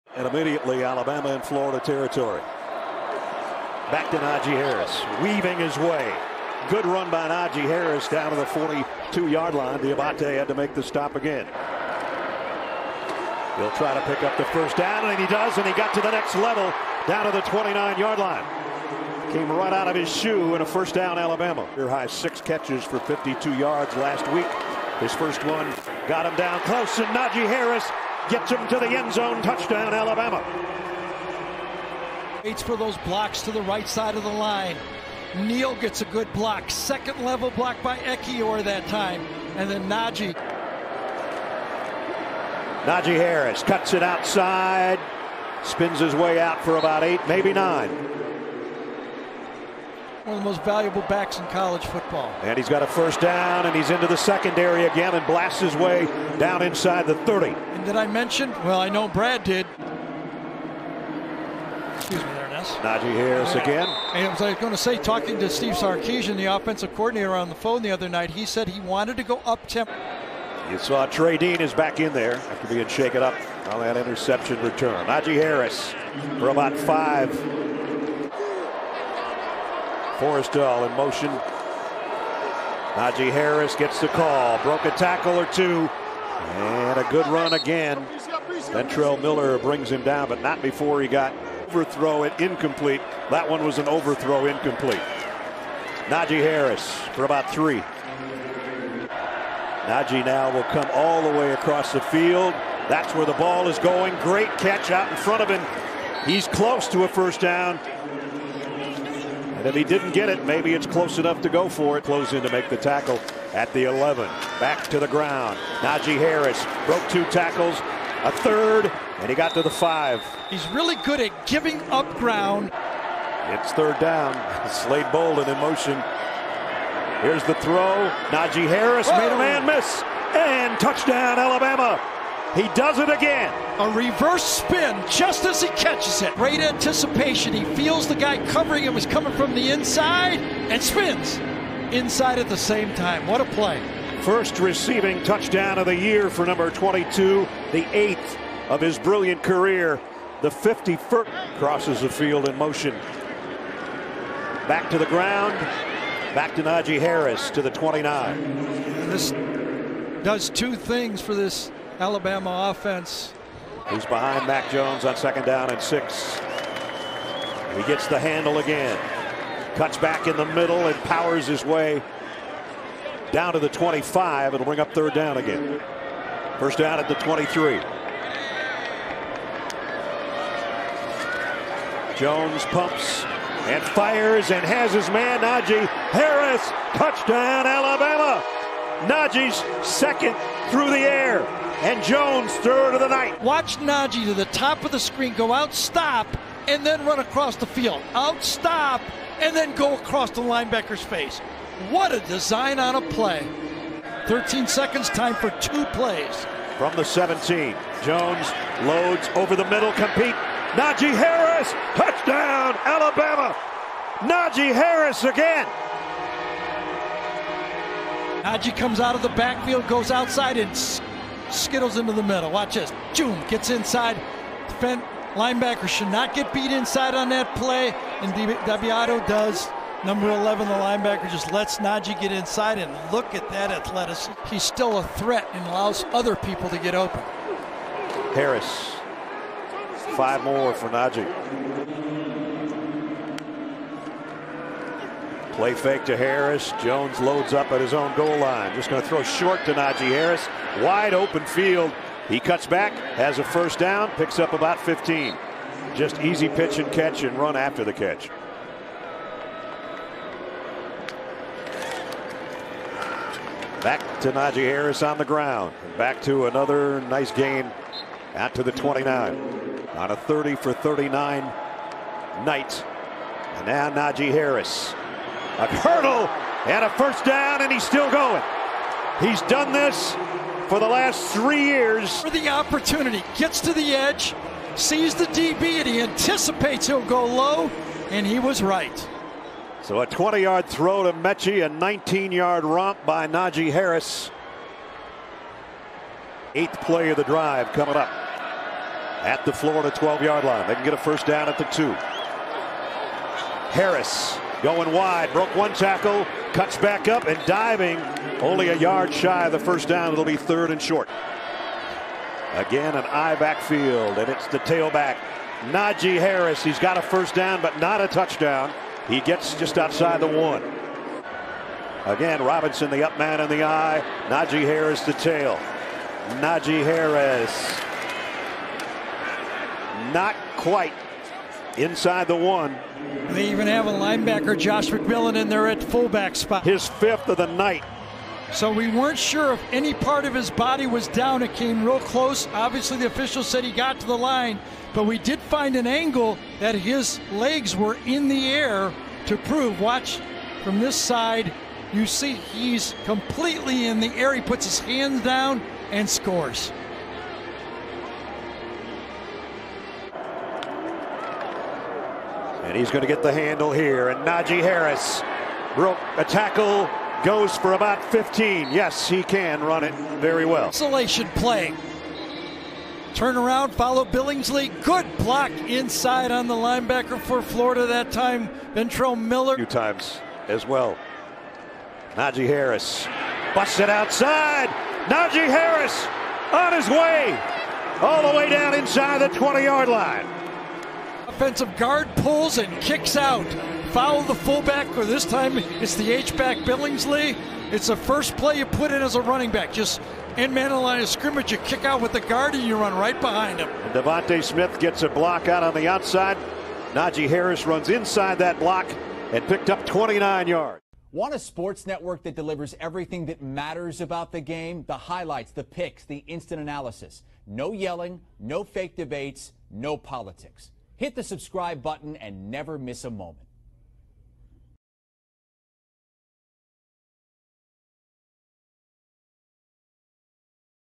Najee Harris Highlights vs. Florida Gators: 2020 SEC Championship | CBS Sports HQ
Running back Najee Harris was the star of the show. The senior totalled 245 yards (178 rushing) with five total touchdowns (two rushing), breaking program records by scoring his 54th overall touchdown and 44th rushing touchdown. His 27 touchdowns in 2020 are one fewer than ex-Alabama running back Derrick Henry had in his Heisman Trophy year in 2015.